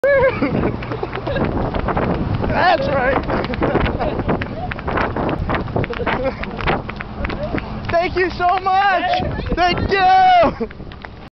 Why does a parrot head cross a 12 lane highway?
To get to the Cheeseburgers in Paradise of course!

So the story behind this is my friends and I took a road trip from Burlington, VT to Charlotte, NC for a Jimmy Buffett concert.  As a place to stay in the middle we picked a motel in Fredricksburg, VA because a Cheeseburger in Paradise, according to the map, was right across the street.  Well turns out this "street" was in fact the first 12 lane highway I've ever been on.

None of us were going to be sober enough to drive 500 yards, and no taxi would take us on that short of a distance, so we asked a car at the intersection to shield us as we crossed the intersection at the next light.

This is that suicidal dash across the street.  We were lucky that the kind waitresses at that CnP took pity on us out of place northerners and we were able to get a ride back home.  We were also luck that they didn't call the cops after "somebody" tried to make off with every utensil, condiment, menu, plate, glass, and shot glass we used that night.  We even got rewarded with free koozies for our kelpto ways.